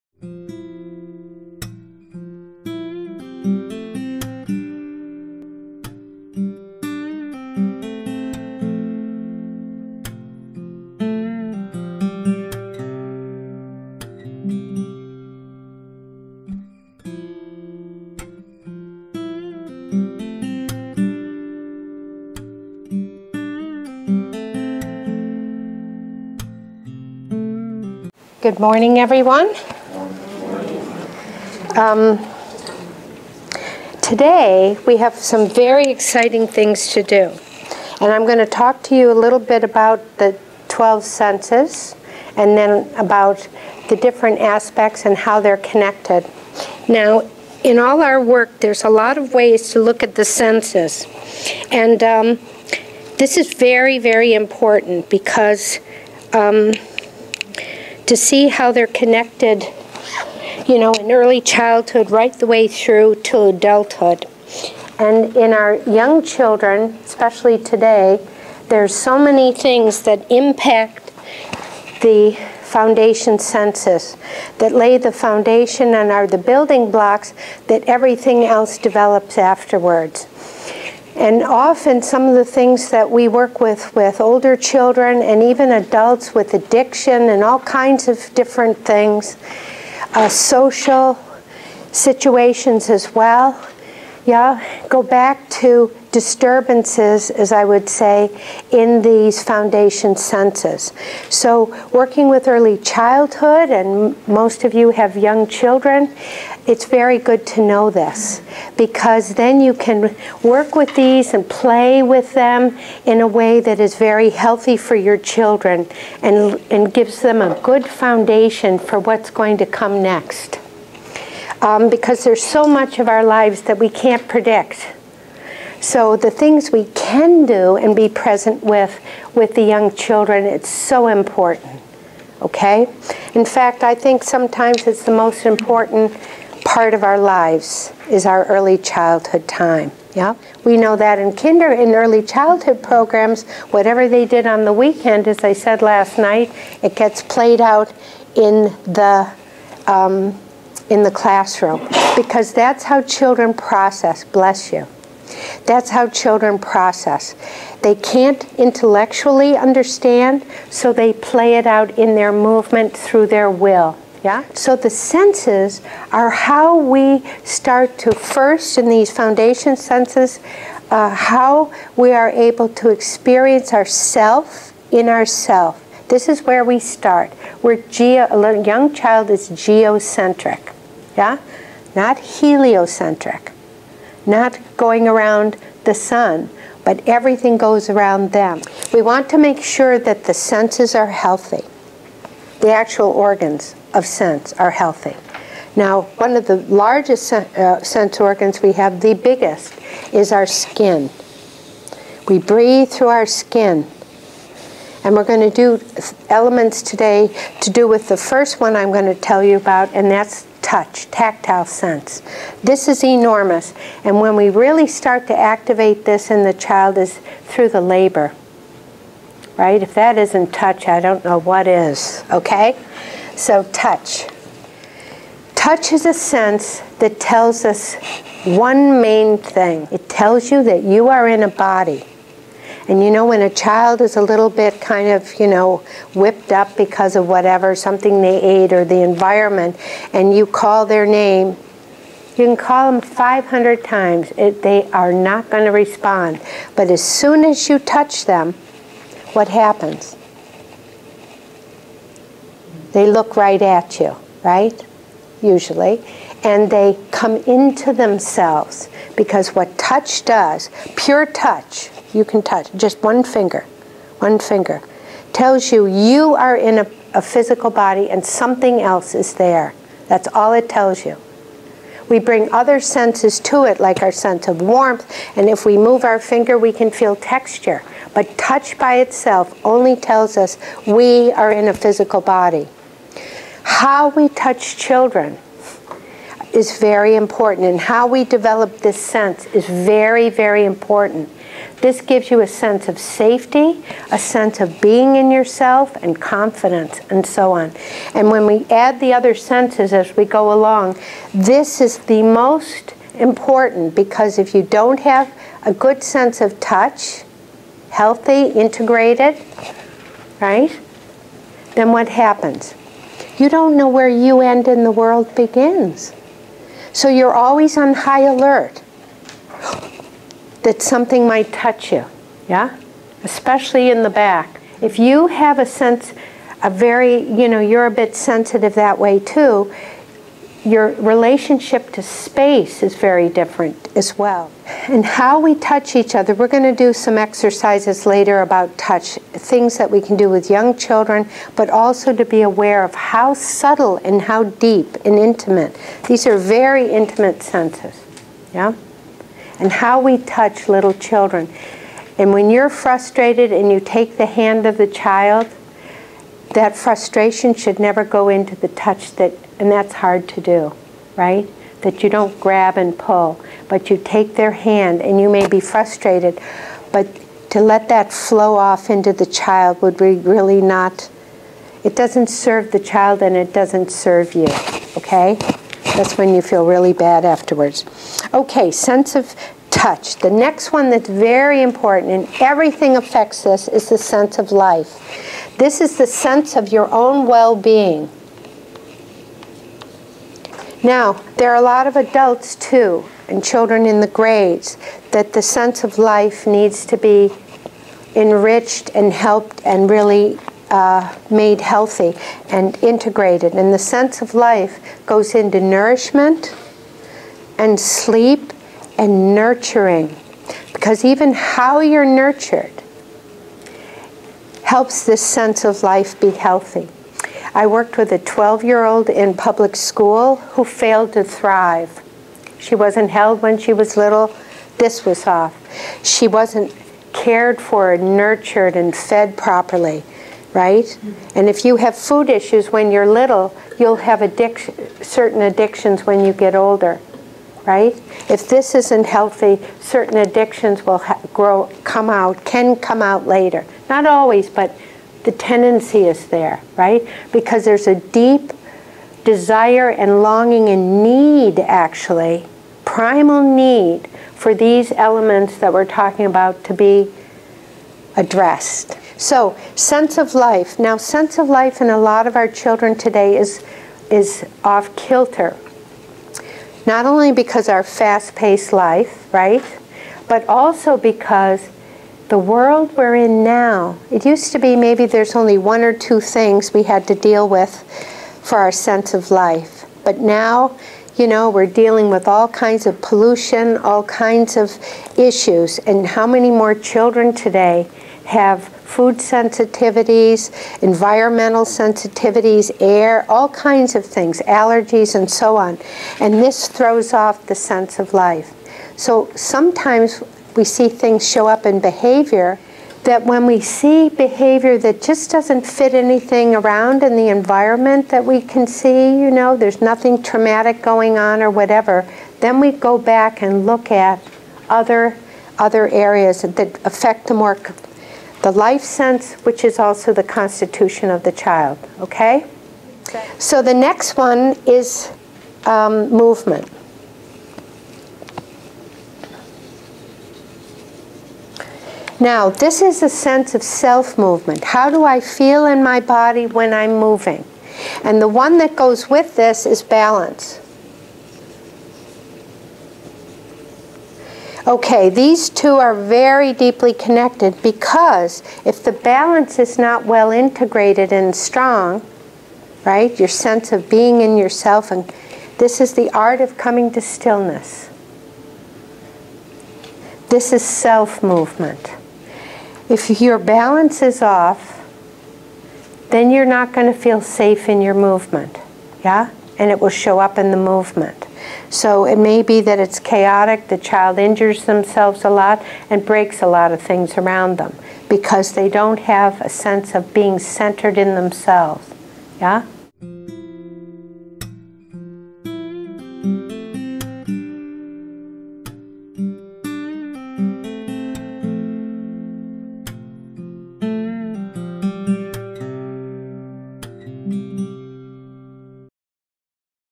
Rachel Ross, M.Ed. - Adventures in Parenting - The 4 Lower Senses
While visiting Lawrence, KS, to teach at the Waldorf Teacher Preparation Course, Rachel Ross, M.Ed., was asked to give a workshop on topics from her book Adventures in Parenting.  Held over a weekend, Rachel treated participants to 4.5 hours in practical parenting considerations, including activities to help children cope with stressors associated with living in today's world.